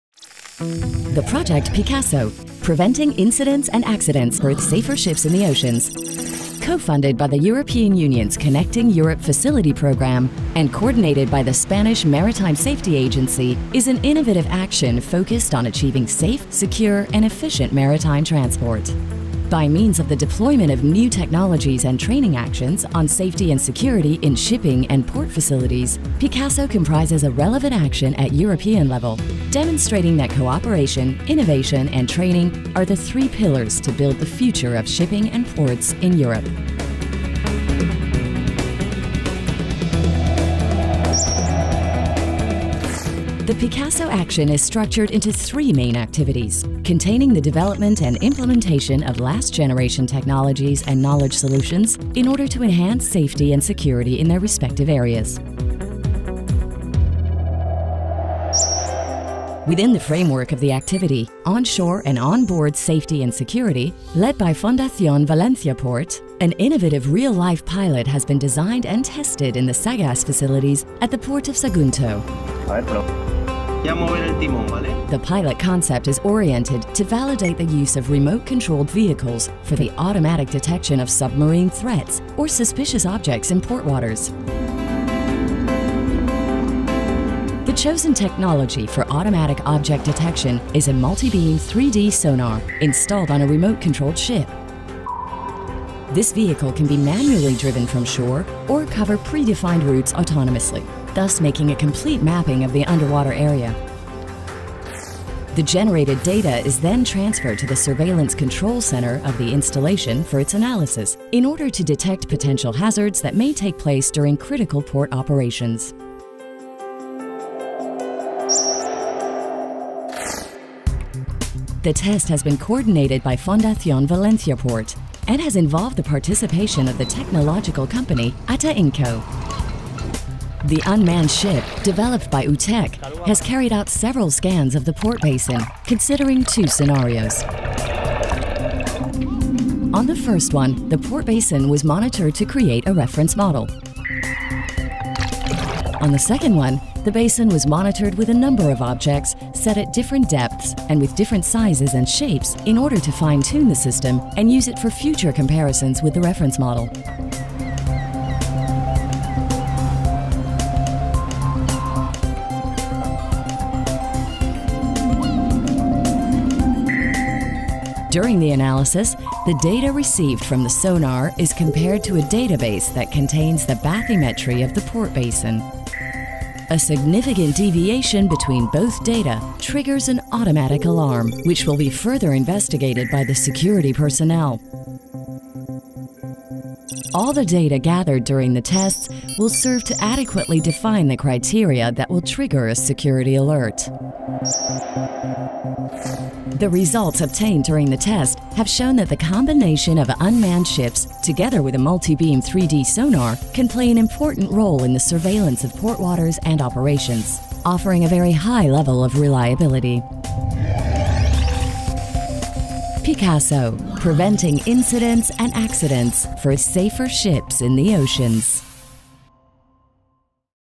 Application of Remote Operated Vehicles for Underwater Port Monitoring and Surveillance.
As an action of the PICASSO project, the Fundación Valenciaport jointly with the technological companies Ateinco and Utek have developed a solution for port safety and security applications, consisting in the integration of Sonar 3D technology on a remote operated vehicle. This concept was tested at the Saggas LNG regasification terminal at the Port of Sagunto. The purpose of the test was to validate the monitoring and surveillance concept through the detection of suspicious objects in the underwater port area using the abovementioned technology.